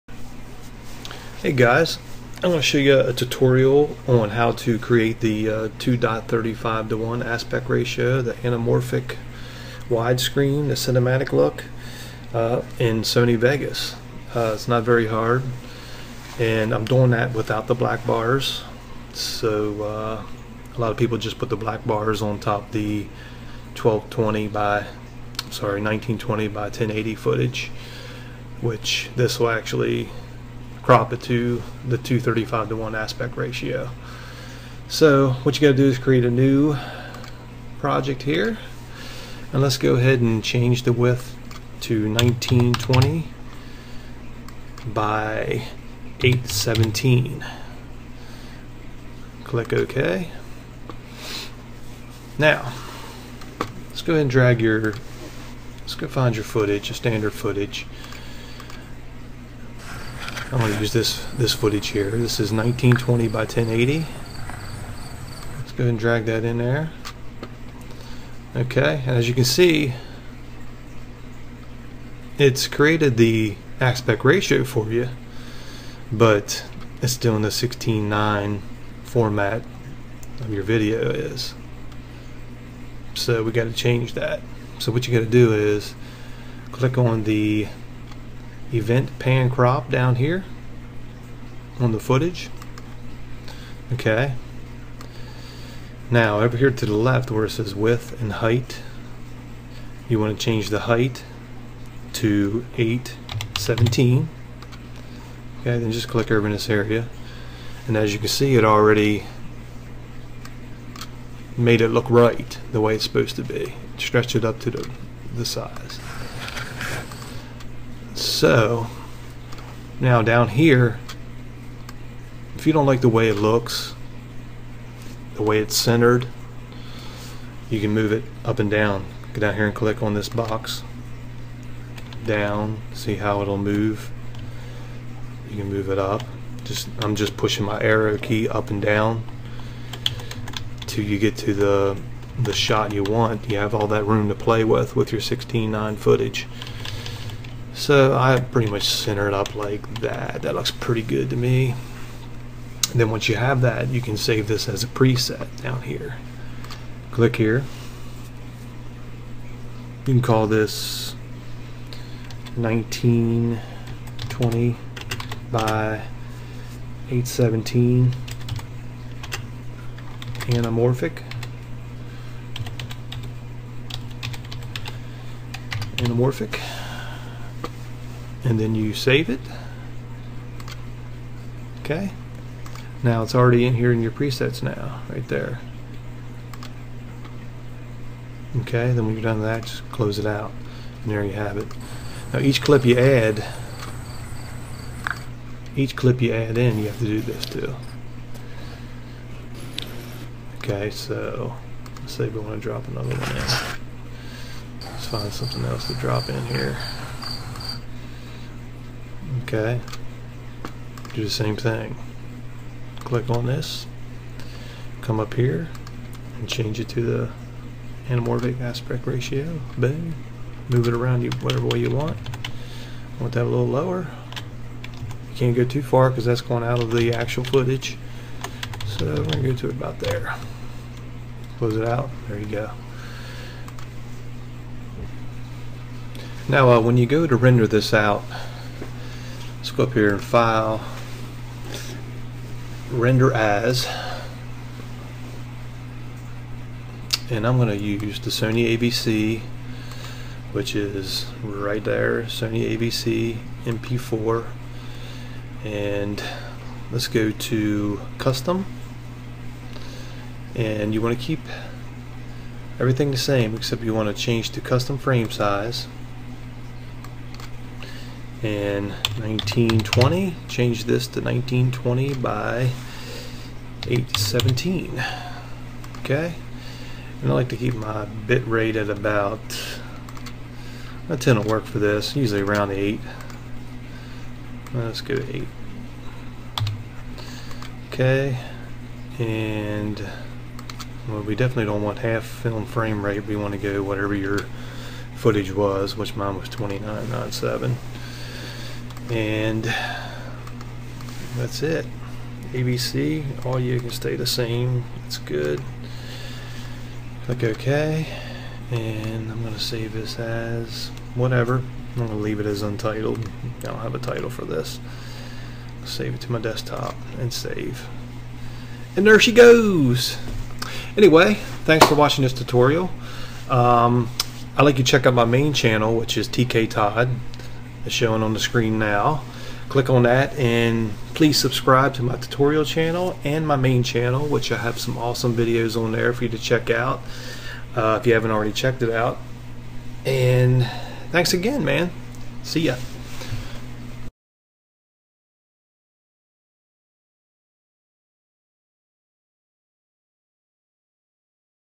Sony Vegas 2.35:1 Aspect Ratio Tutorial | No Black Bars
I know the title may be a bit misleading. When I say NO BLACK BARS, What I mean is that it will not show black bars when uploaded to sites like vimeo or when played back with quicktime player when not in full screen. That is the only benefit to this tutorial. If you are playing it back on a 16:9 TV, you are still going to see black bars. Thanks!!